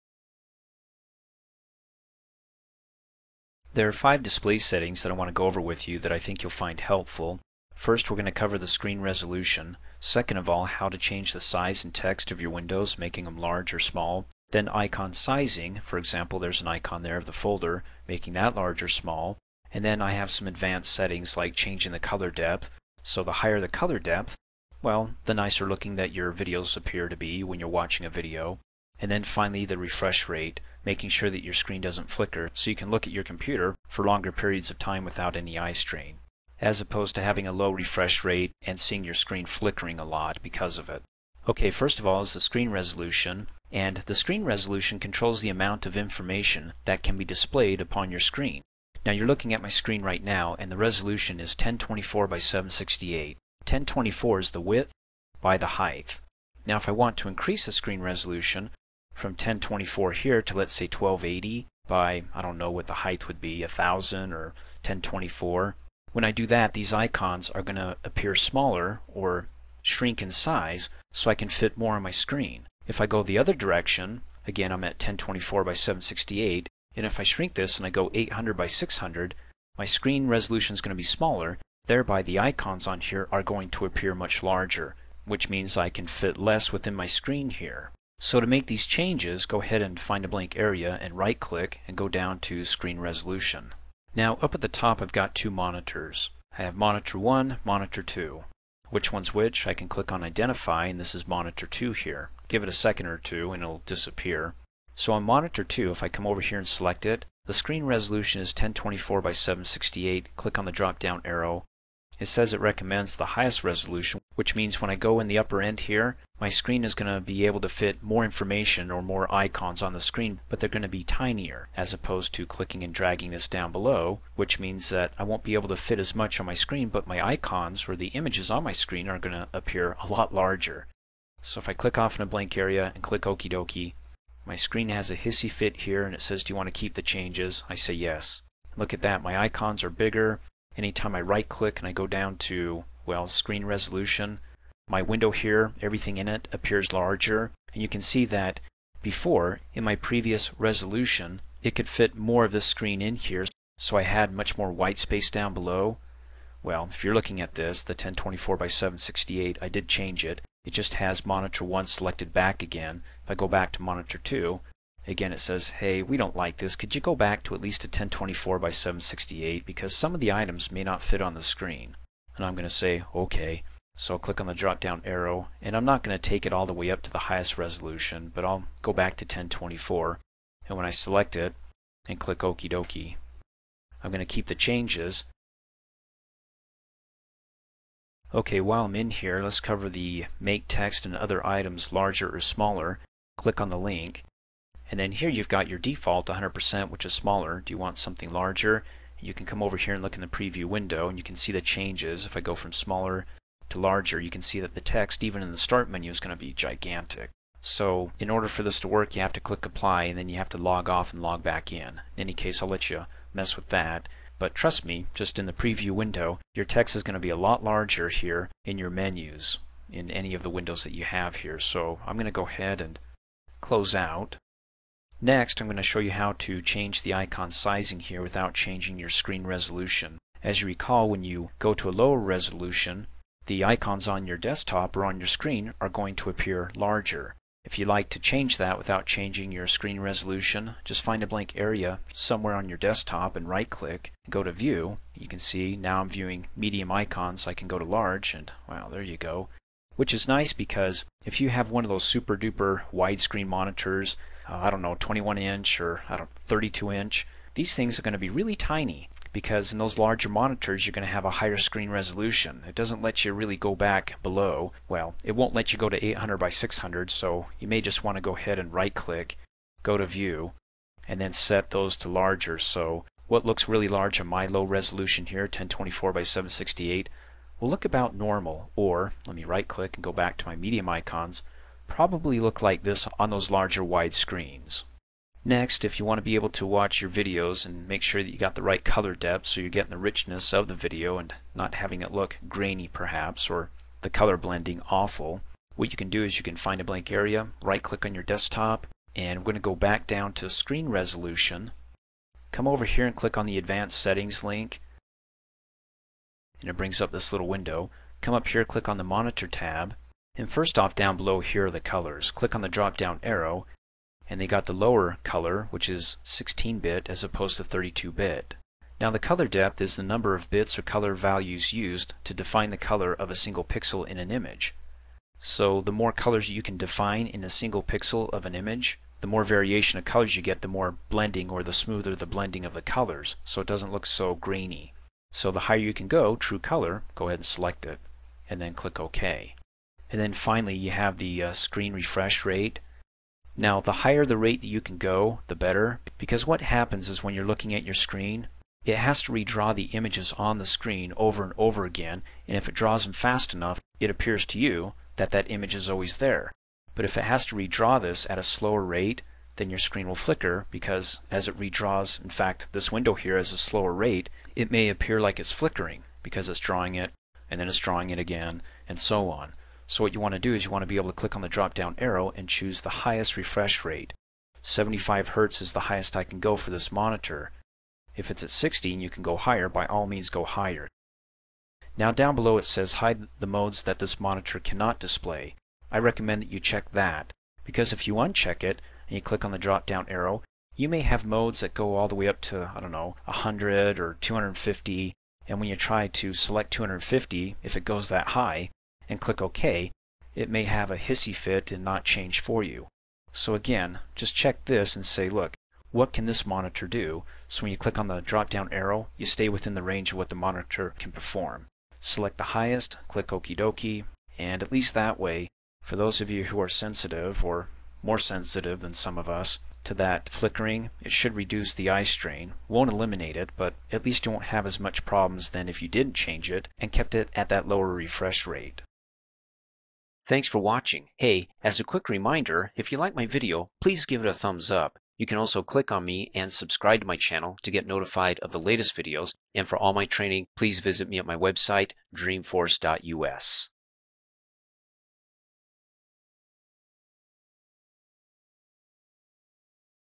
Windows 7: Top 5 Windows Display Settings You Can Change
Windows 7 training video on the top 5 display settings including: screen resolution, change size and text of Windows, change the size of your Desktop icons, changing the color depth for more captivating videos and the refresh rate - to avoid screen flickering and being able to look at your screen longer with less eyestrain and headaches.